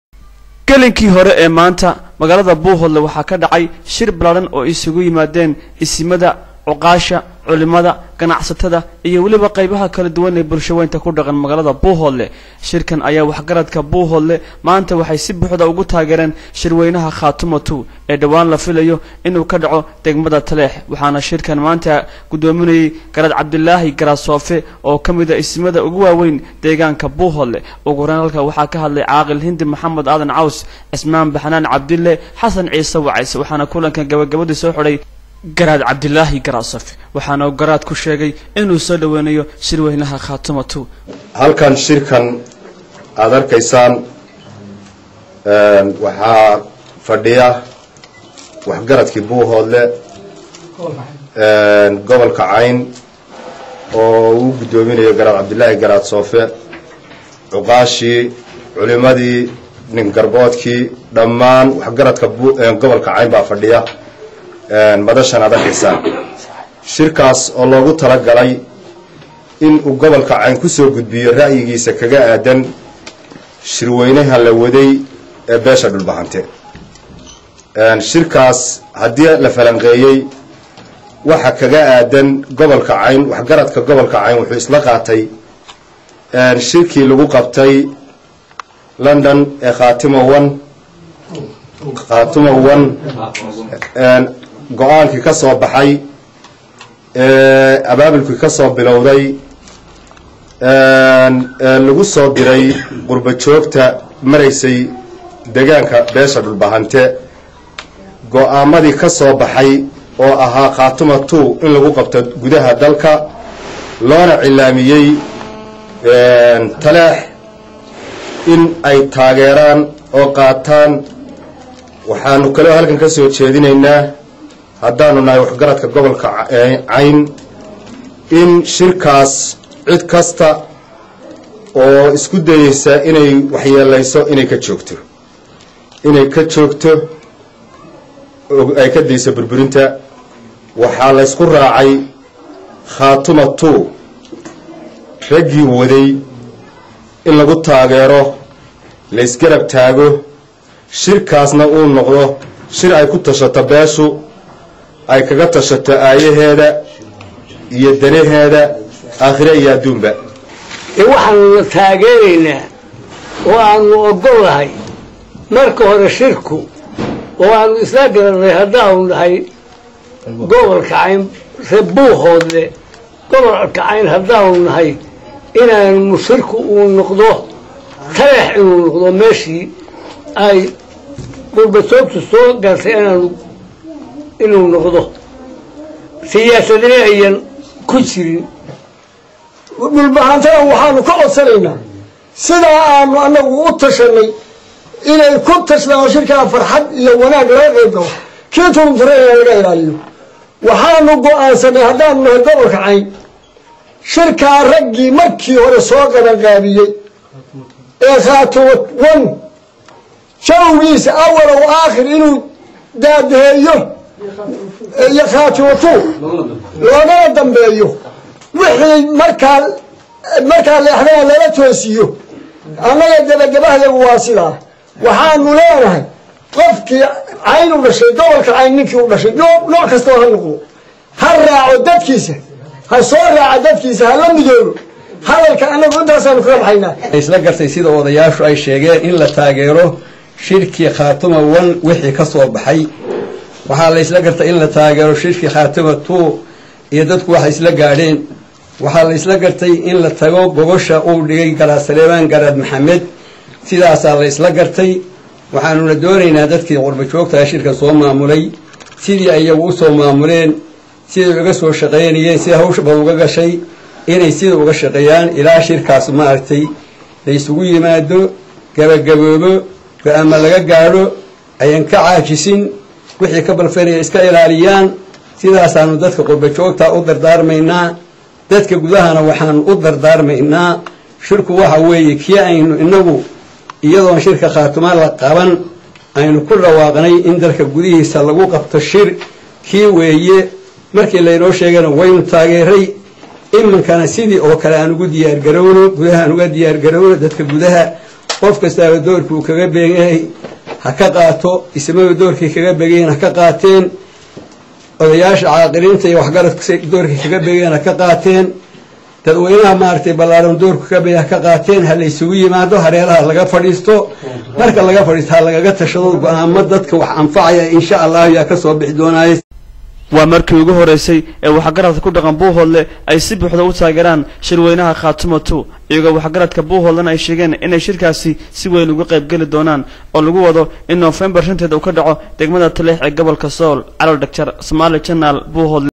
Waxgaradka Buuhoodle oo taageeray Shirka Khaatumo2.wmv
Waxgaradka Buuhoodle oo Garaad Abdilahi  Garaad soofe Hogaaminayo oo taageeray Shirka khaatumo2